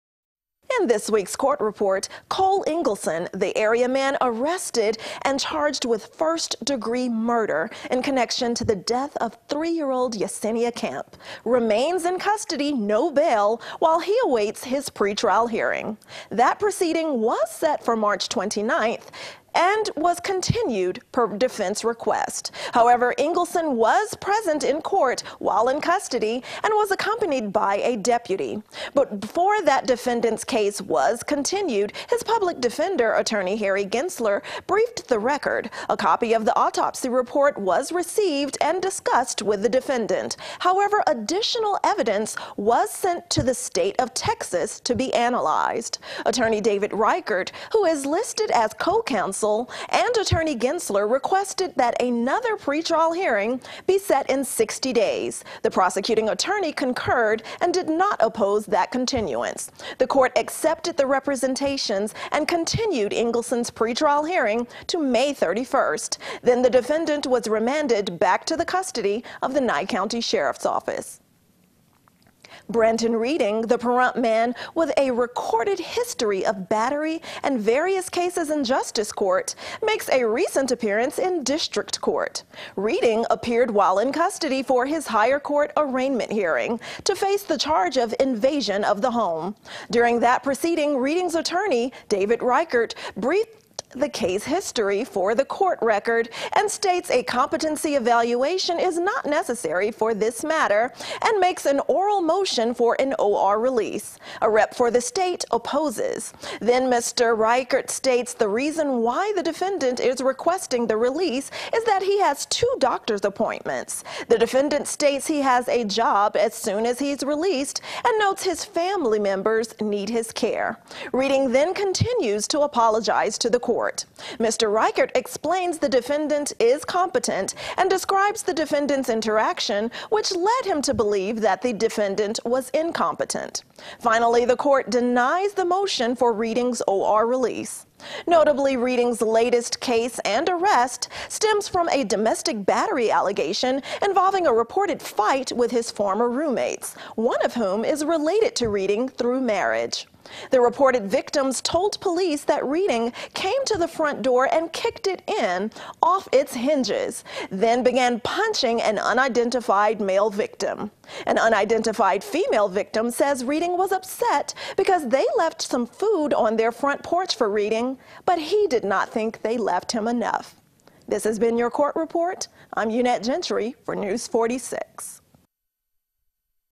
04/17/2018 Court Report | Eunette Gentry
Here's Eunette with the latest on what's happening in our courts.